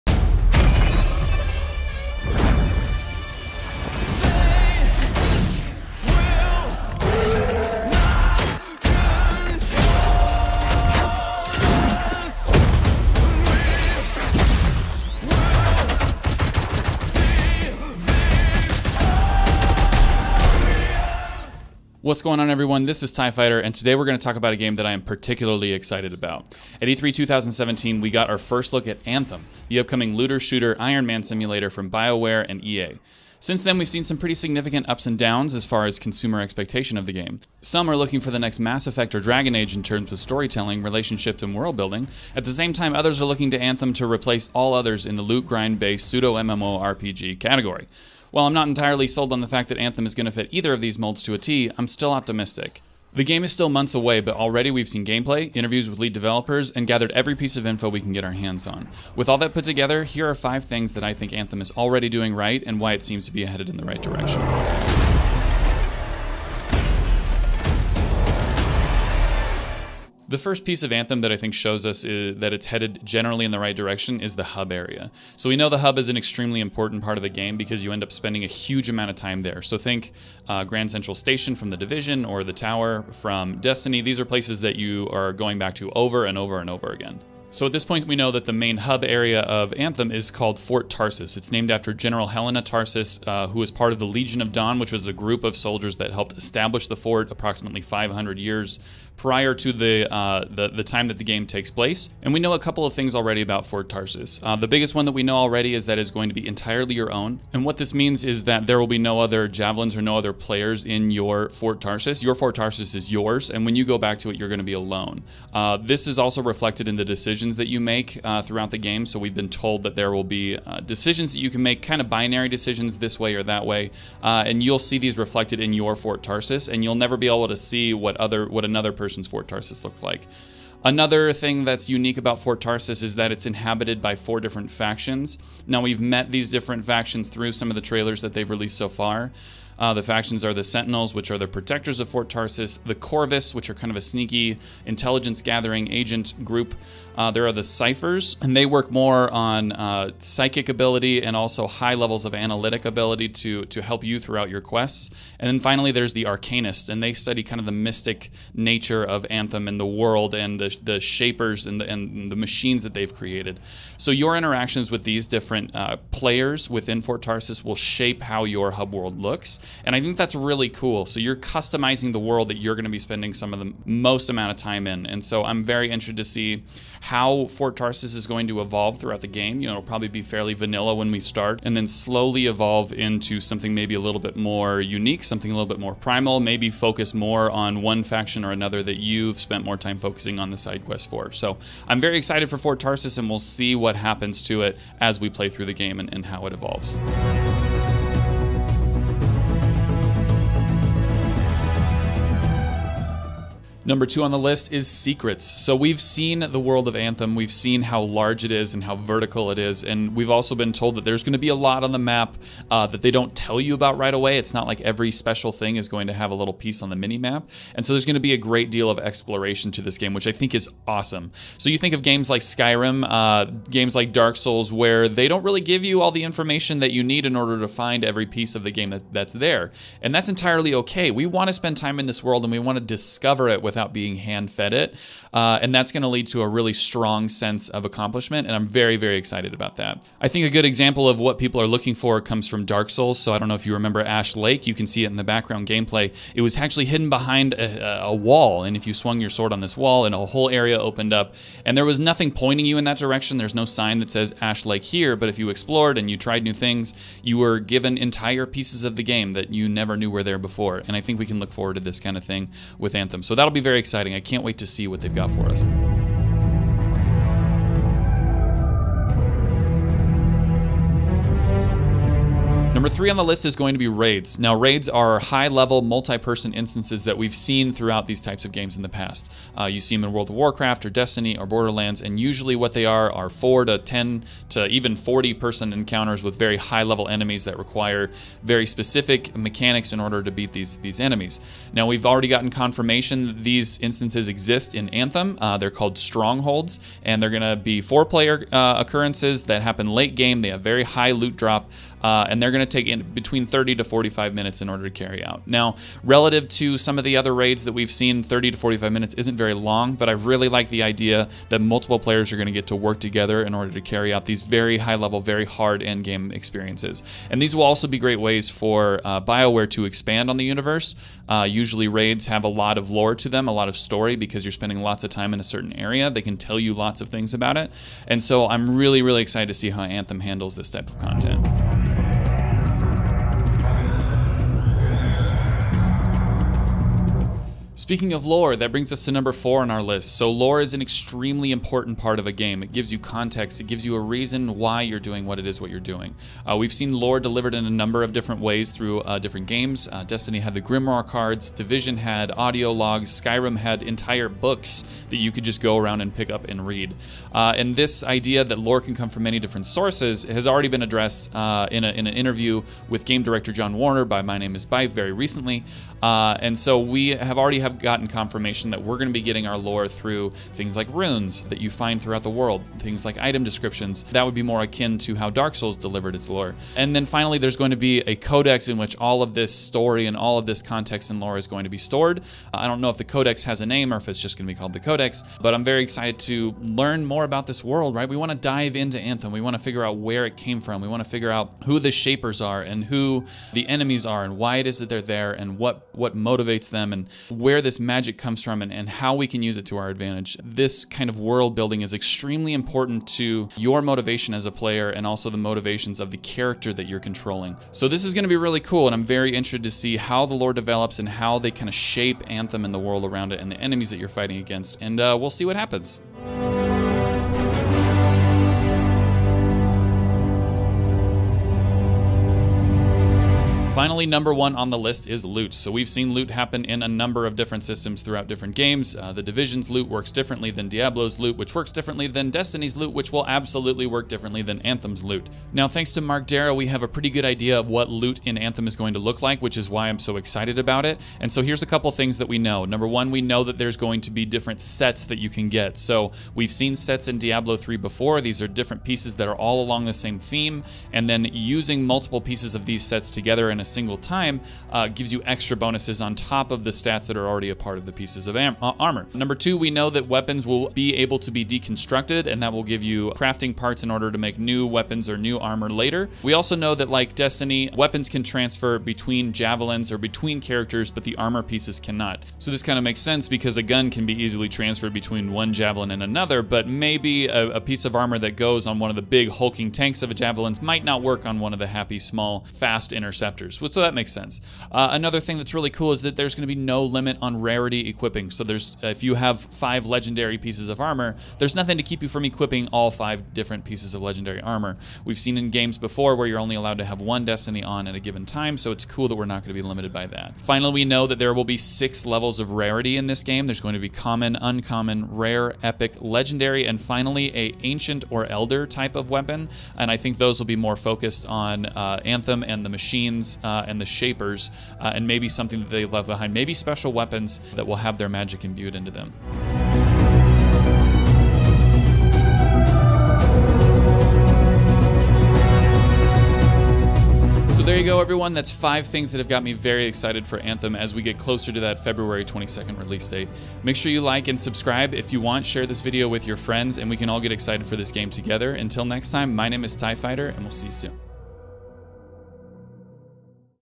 5 Things Anthem Is Already Doing Right
I couldn't be more excited to play Anthem! The loot, the lore, everything looks amazing!

Anthem is an open world third person looter/shooter set in an alien world deep in the future. Players will use hyper-powerful suits called Javelins to explore the land and defend themselves from the many dangers that await them there.

Bioware and EA have already advertised Anthem as a shared world but a personal story. Players can drop in and out of each other's instance in order to team up to defeat strong enemies and encounters, but will also have the ability to experience the game and its story alone. Choices will be made that affect the players experience and how they perceive their world.

The demo arrives on Febuary 1, 2019 for all EA Access and Origin members as well as everyone who pre-ordered the game. Final release will take place three weeks later on Febuary 22nd. I couldn't be more excited to see what awaits us in this mysterious, unfinished, and violent world!